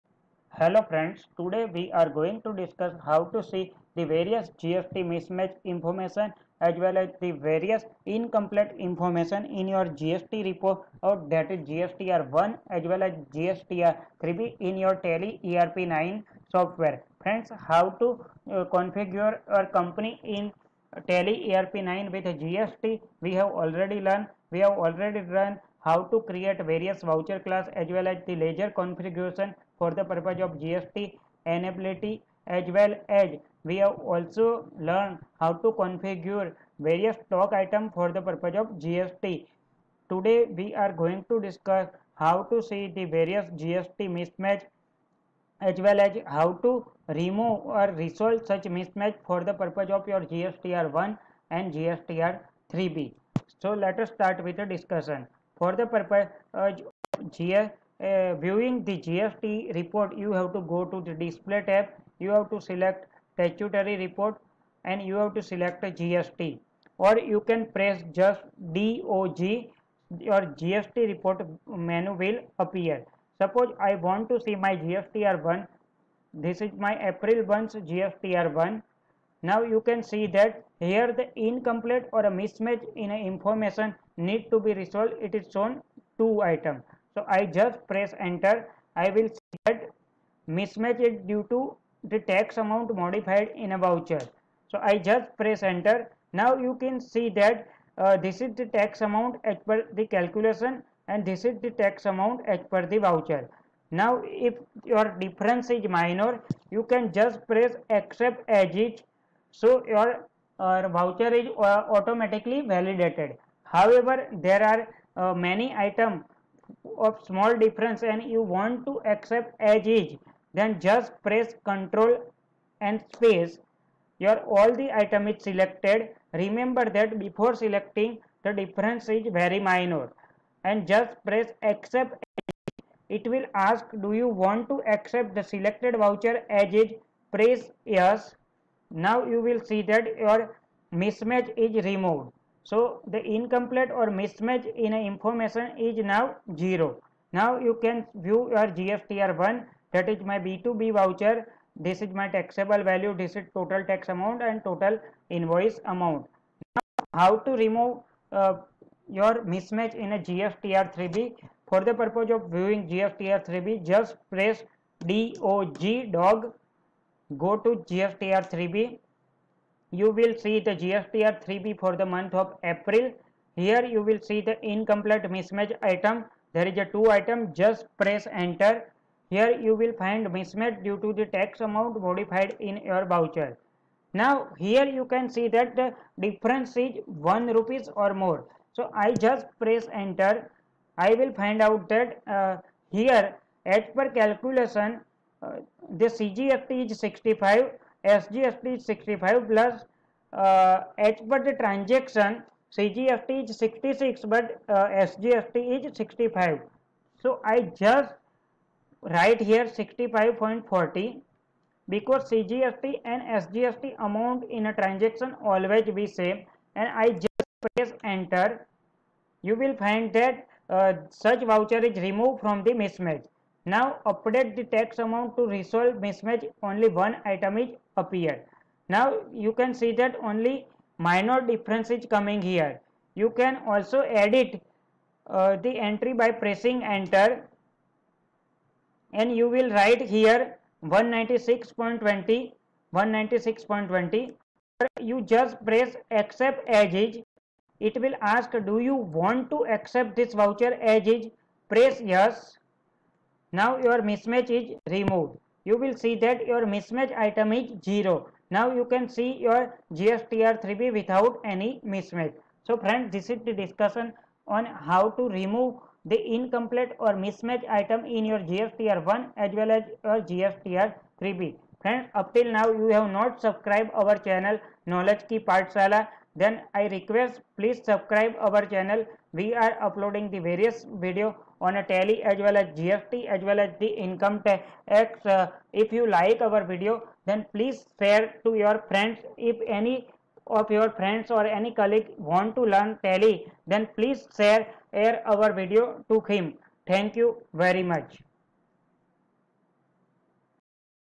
REMOVE MISMATCH IN GSTR1 AND GSTR3B IN TALLY - Easy way to solve the mismatch in GST Report in tally
The video is prepared to enable the viewers to get information about the manner in which mismatch in gst reports in tally ERP 9 is solved.